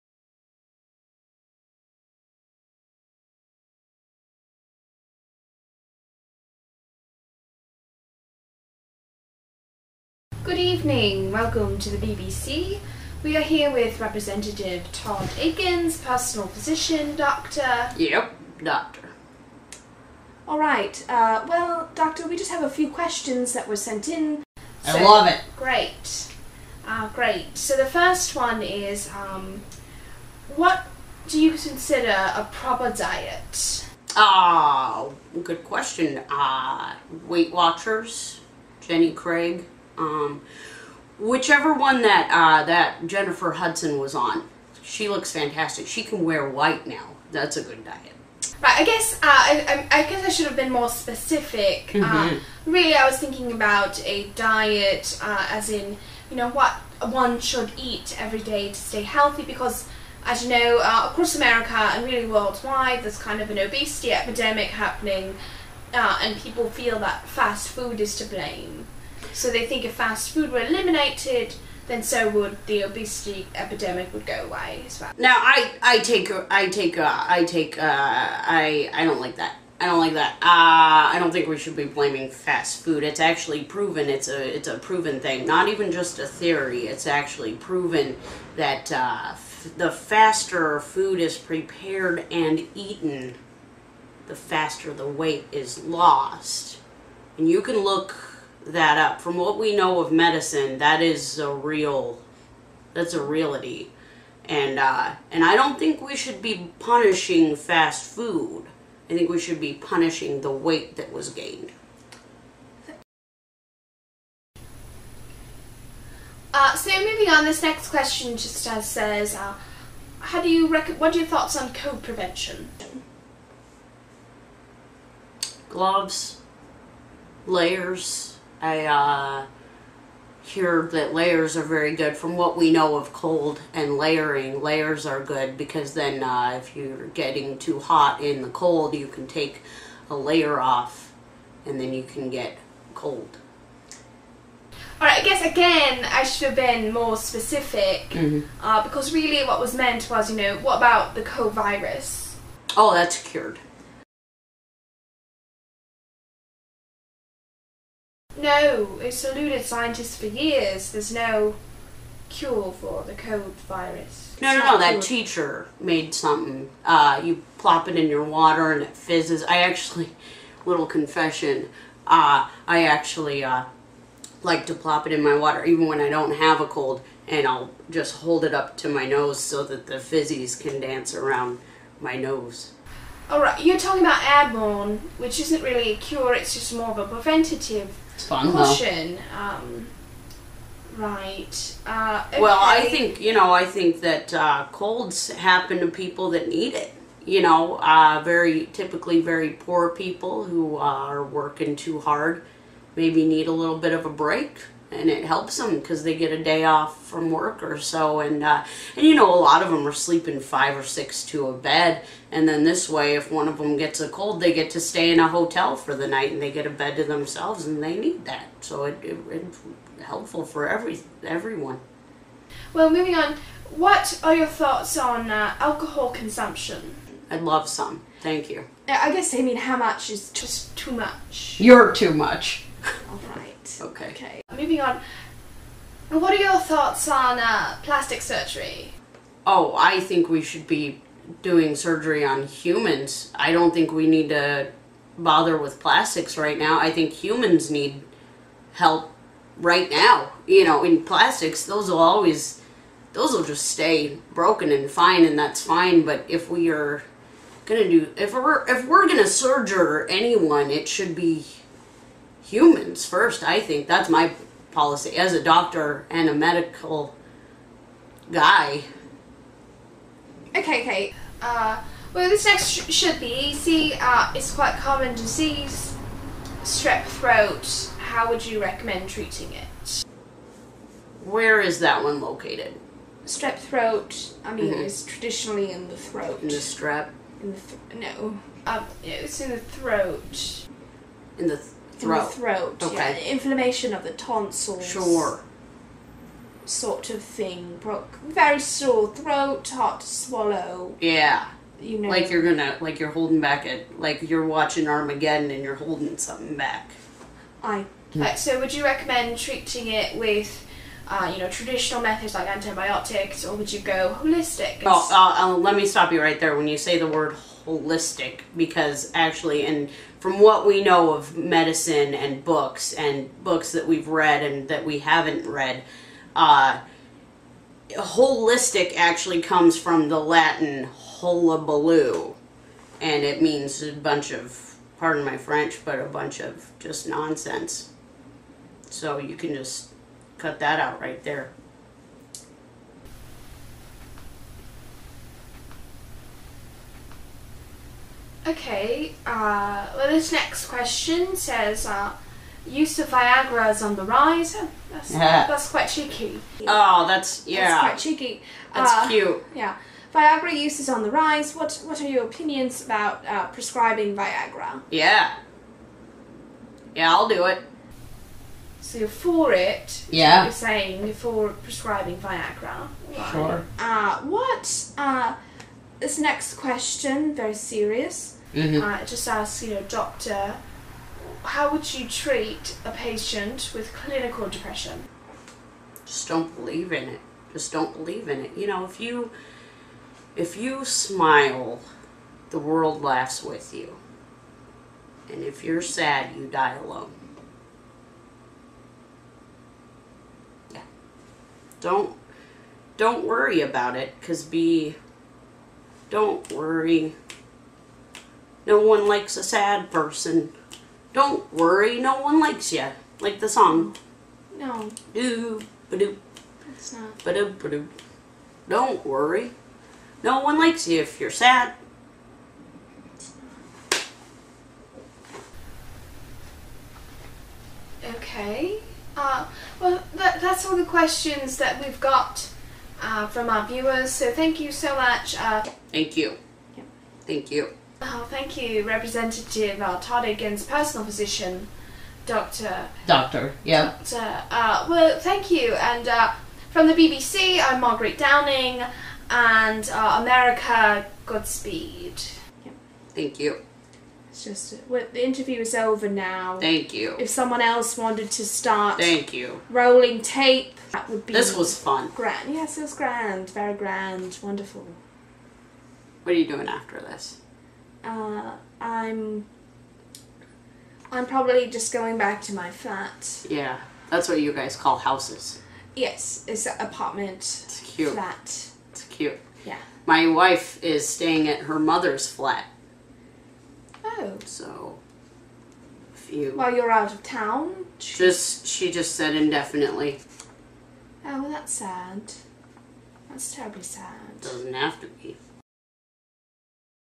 Todd Akin's Doctor's medical advice
Todd Akin has heard some interesting facts from medical professionals. We caught up with one of his doctors to see what "Doctor" has to say about other medical issues.